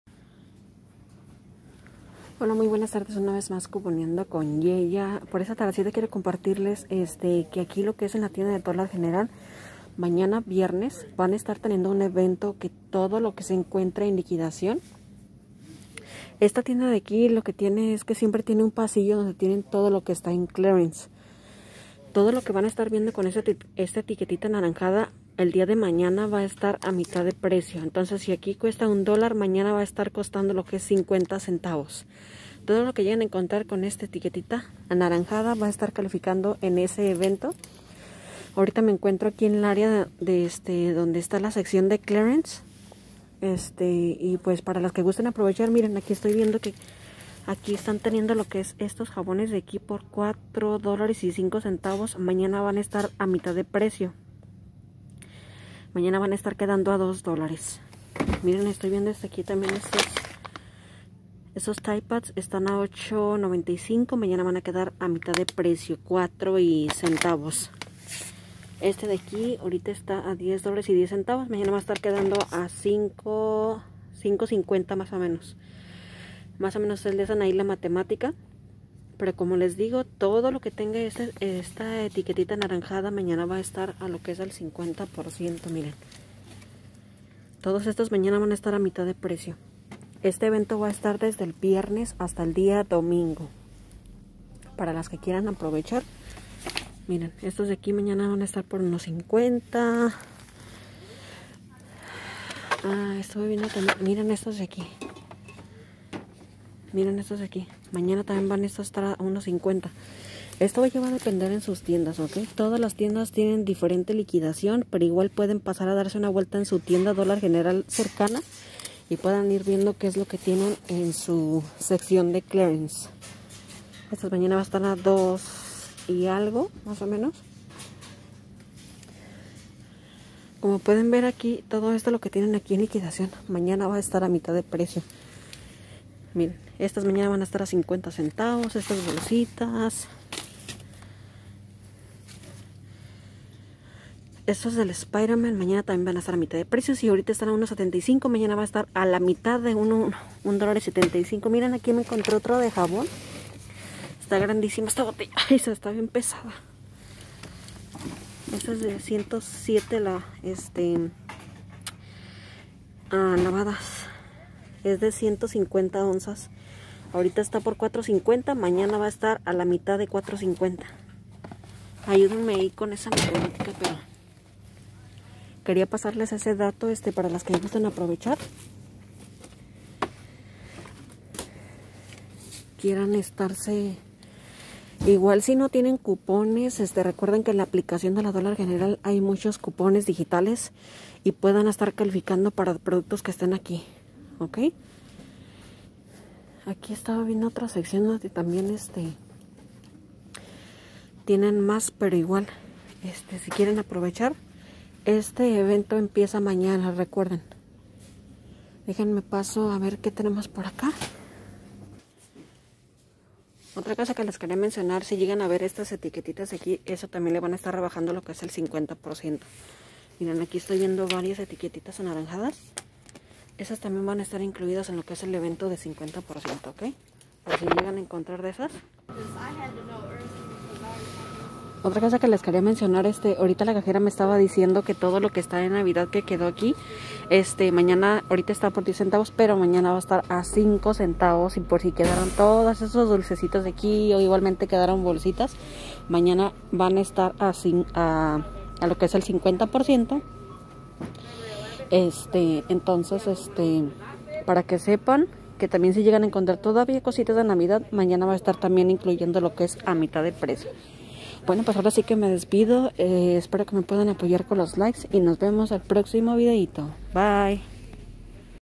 Evento de Dollar general del 29- 31 de Enero 👌👌👌👌
Ofertas vigentes del viernes hasta el domingo 31 De enero.